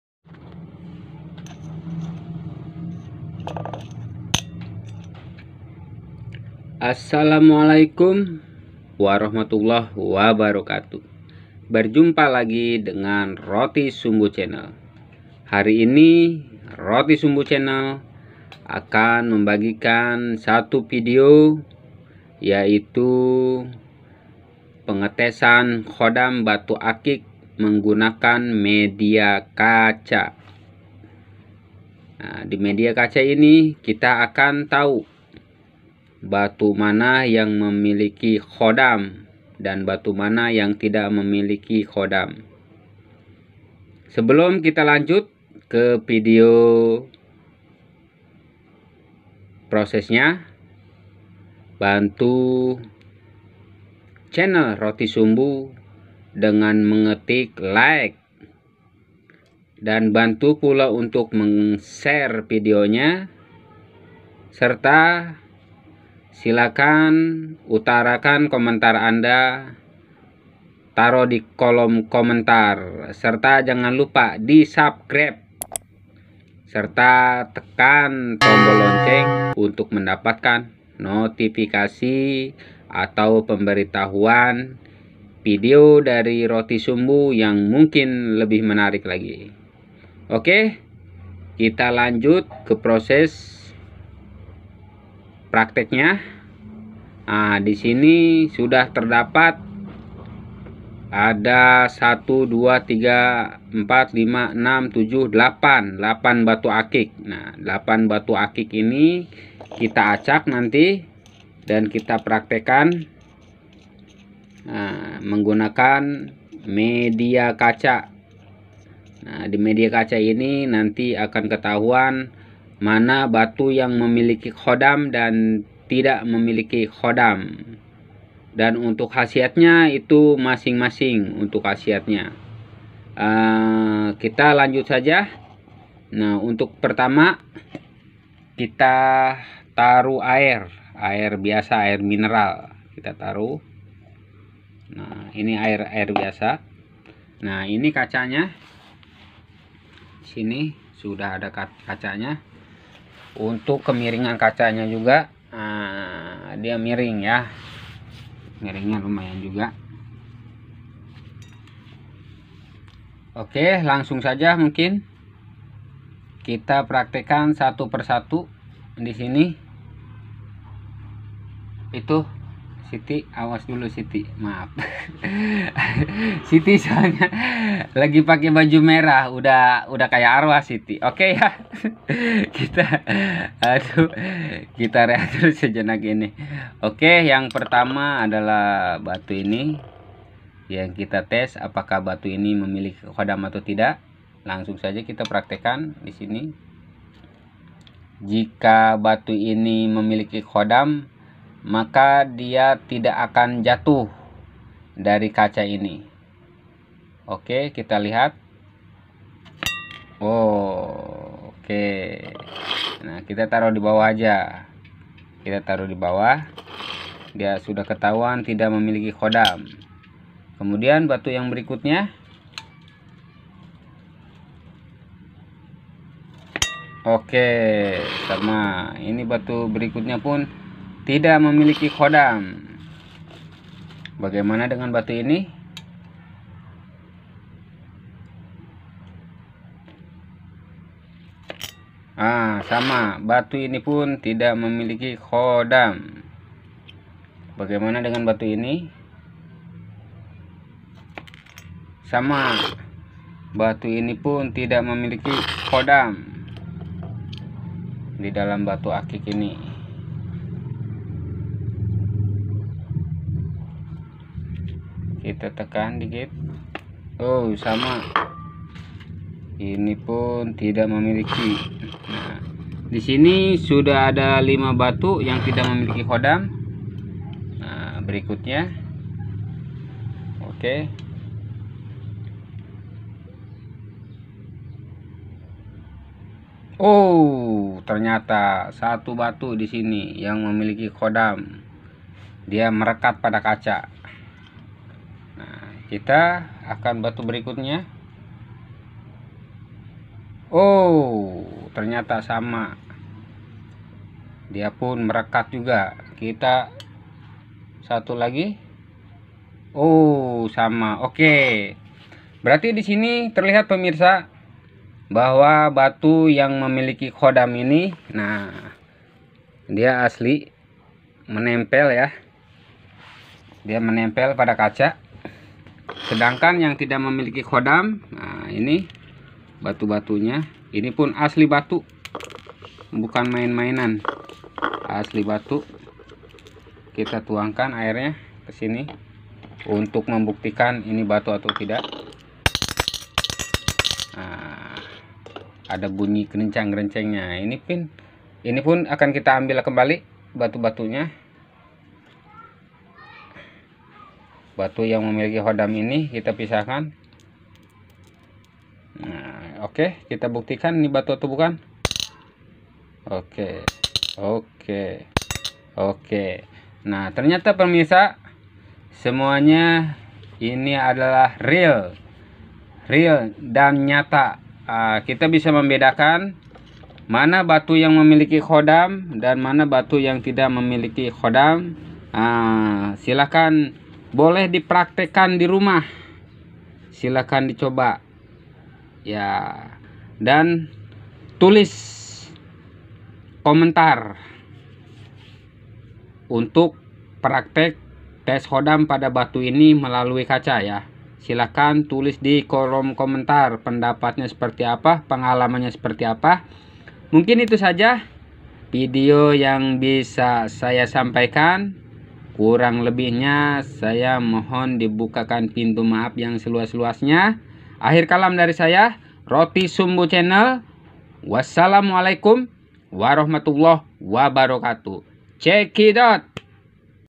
tes khodam batu akik dengan media kaca
batu akik adalah suatu bentuk proses alam yang sangat menarik dan indah dan batu akik memiliki suatu daya tarik dan kharisma tersendiri.
Dan batu akik memiliki beberapa khasiat kegunaan karena di percaya oleh sebagian orang bahwa batu akik memiliki suatu khodam atau kekuatan.
dan di video ini saya akan memberikan cara pengetesan batu akik yang berkhodam ataupun tidak dengan media kaca dan untuk caranya simak video ini sampai selesai lalu praktekkan dan buktikan.
terlepas dari semua itu, tergantung kepada tanggapan sahabat @rotisumbuErikGemstone channel percaya atau tidak percaya.